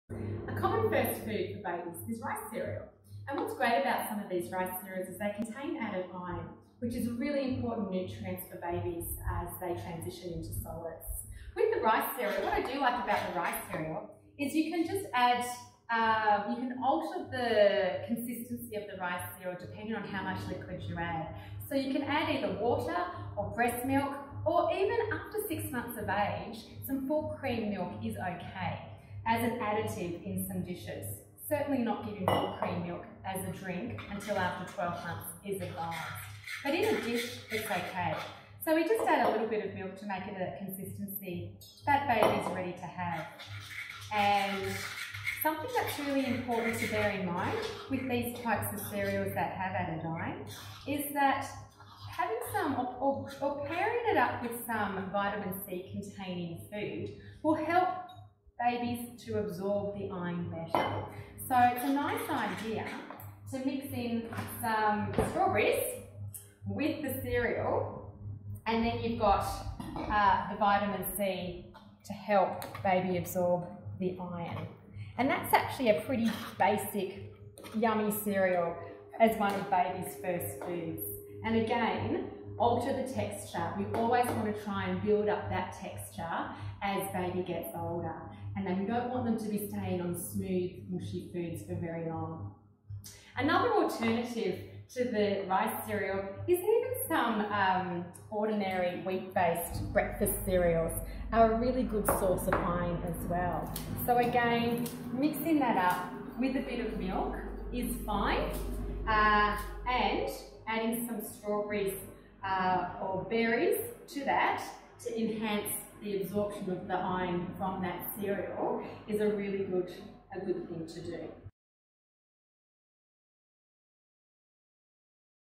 Iron fortified cereals
Iron fortified cereals can be a great first food for babies. Dietitian, Leanne Elliston, demonstrates how you can prepare the cereal for your baby..